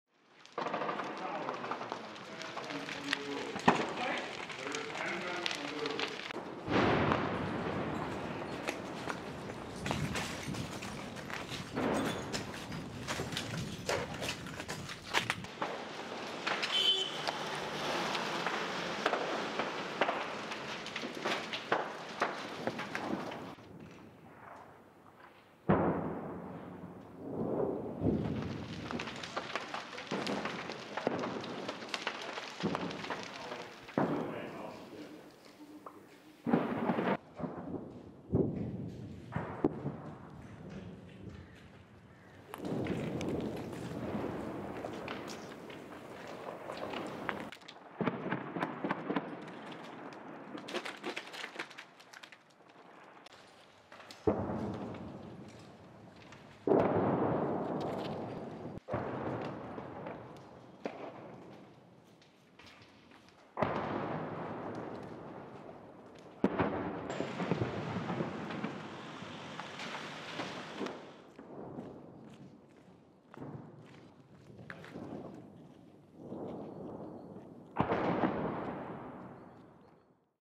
Flăcările au cuprins orașul Bahmut în timp ce atacul rusesc se intensifică
Videoclipul surprins luni (27 februarie) arată pagubele cauzate clădirilor de loviturile din orașul Bahmut din prima linie, în regiunea Donețk din Ucraina.
Locuitorii din zonă pot fi văzuți mergând pe străzi pe lângă clădirile distruse, în timp ce în fundal sunau explozii puternice și lupte de armă.
Forțele ruse au continuat marți (28 februarie) drumul lor început de câteva săptămâni pentru a încercui și captura orașul Bahmut, unde comandantul forțelor terestre ale Ucrainei a descris situația ca fiind „extrem de tensionată”.
Trupele ruse, inclusiv luptători mercenari din Grupul Wagner, încearcă să taie liniile de aprovizionare ale apărătorilor ucraineni către oraș, locul unora dintre cele mai sângeroase bătălii ale războiului, și să îi oblige să se predea sau să se retragă.